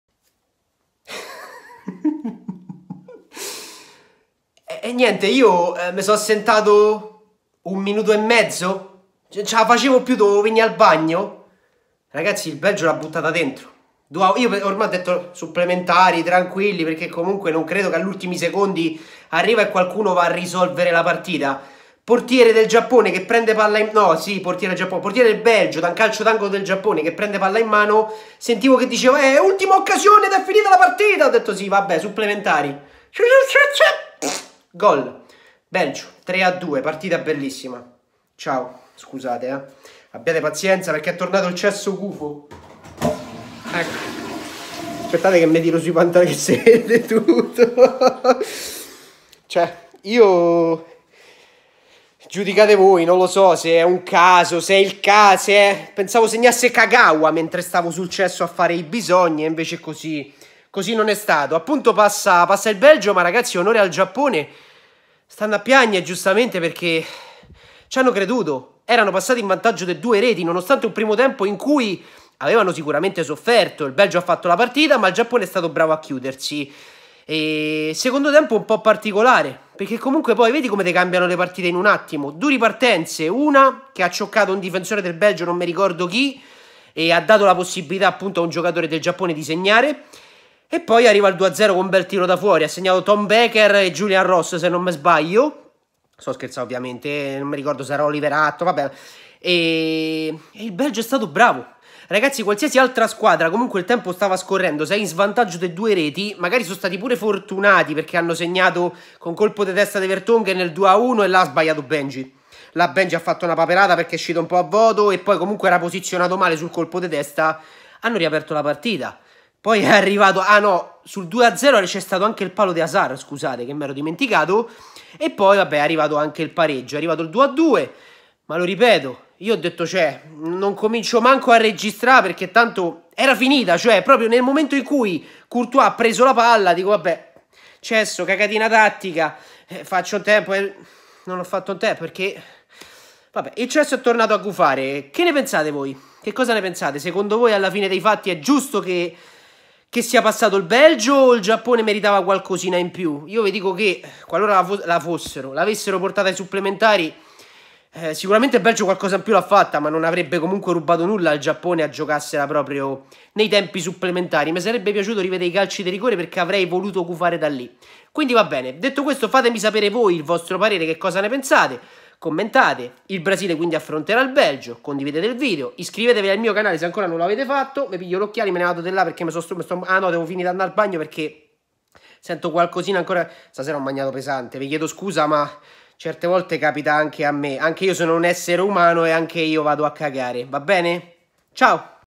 All’ULTIMO SECONDO!!! Belgio-Giappone 3-2!!!
Belgio-Giappone 3-2 ALLO SCADERE!!! Vi dico il mio parere in questo nuovo post partita-reazione dei Mondiali di Russia 2018, come sempre in compagnia di Gabboman!!!